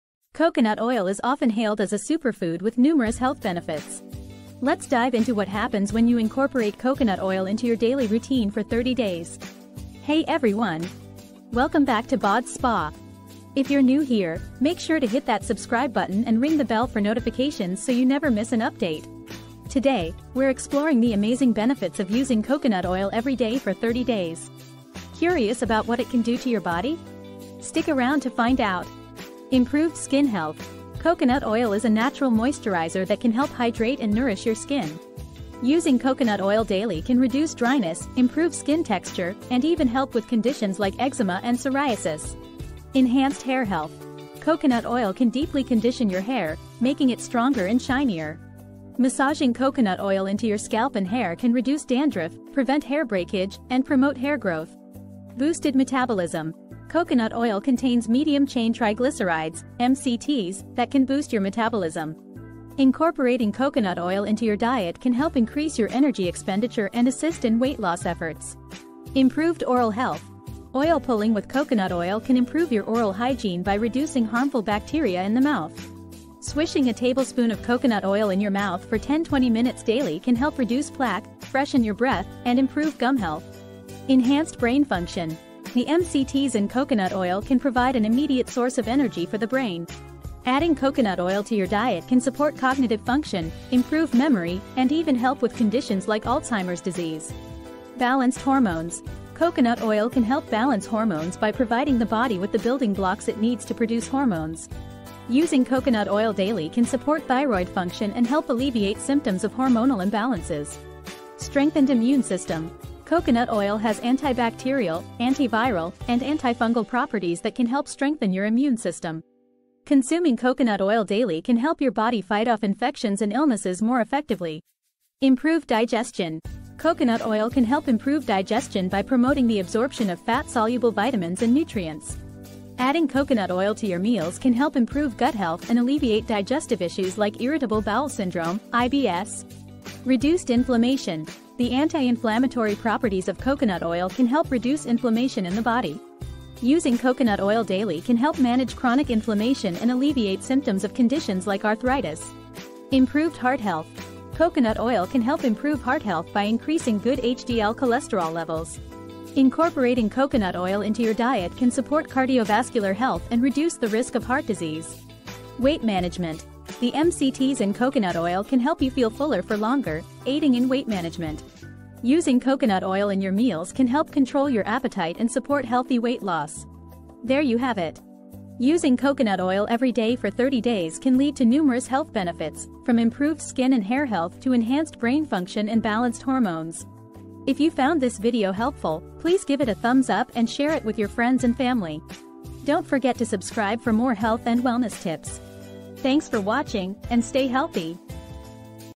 Use Coconut Oil EVERY DAY For 30 Days, See What Happens To Your Body
Discover the incredible results of using coconut oil every day for 30 days in this mind-blowing transformation video!
Watch as the body goes through a remarkable journey and see the amazing changes that occur. From improved skin and hair to increased energy levels, the benefits of coconut oil are truly astonishing.
Don't miss out on this eye-opening experiment and consider incorporating coconut oil into your daily routine for a healthier and happier you!

Other videos recommended for you:

🎥WATCH :